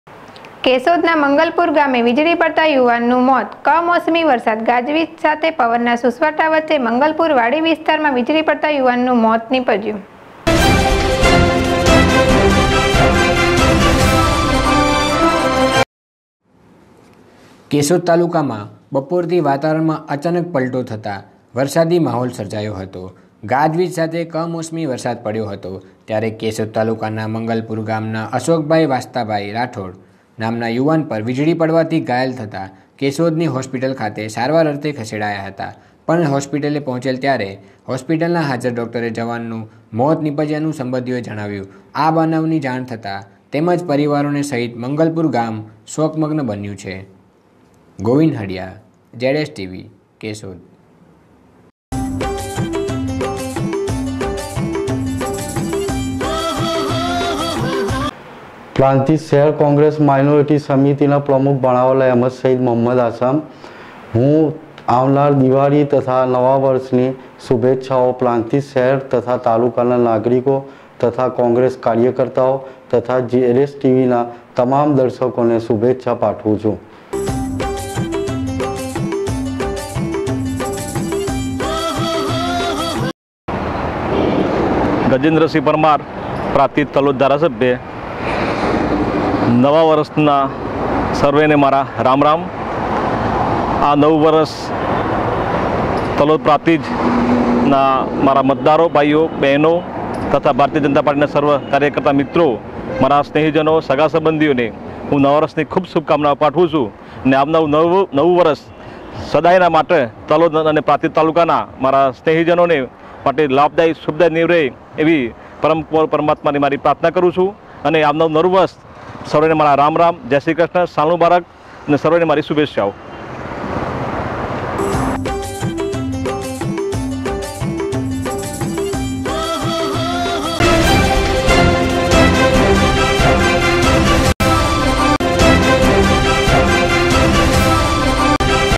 કેશોદના મંગલપુર ગામે વિજળી પડતા યુવાનનું મોત
કેશોદના મંગલપુર ગામે વિજળી પડતા યુવાનનું મોત કમોસમી વરસાદ ગાજવીજ સાથે  પવનના સુસવાટા વચ્ચે મંગલપુર વાડી વિસ્તારમાં વિજળી પડતા યુવાનનું મોત નીપજ્યું